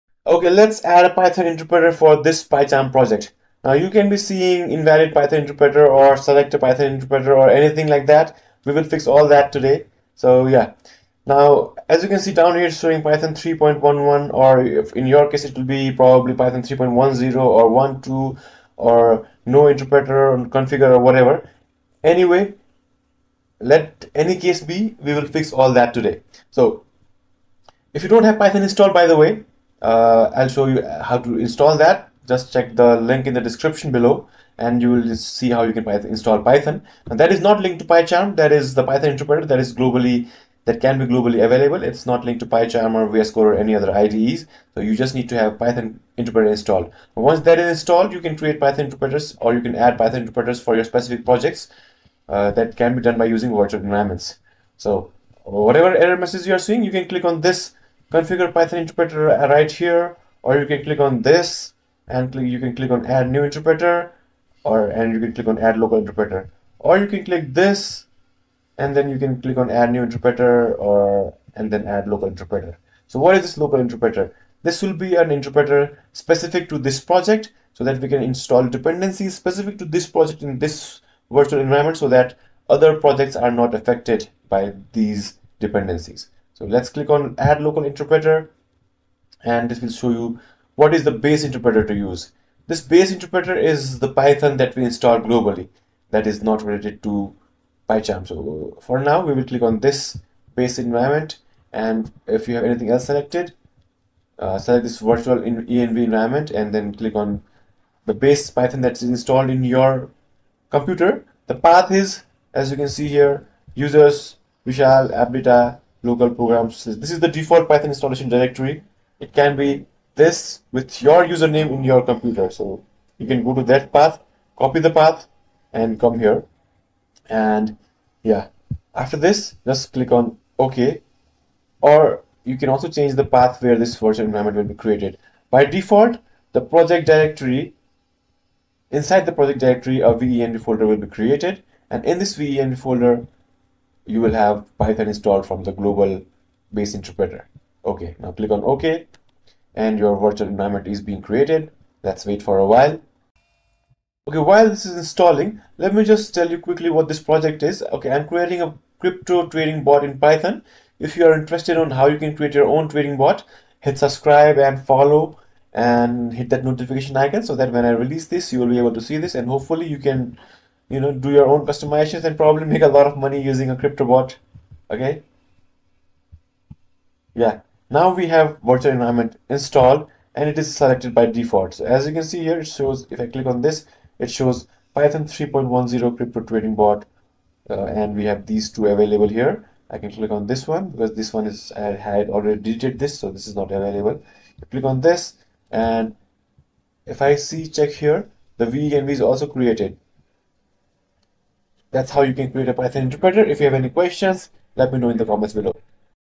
How to Create Virtual environment in PyCharm for Projects (2023 Update)
PyCharm is a very popular IDE for coding in Python. In this video, I'll show you how you can create virtual environment for your project in pycharm. Virtual Environment helps you to manage dependencies and required packages within the project directory.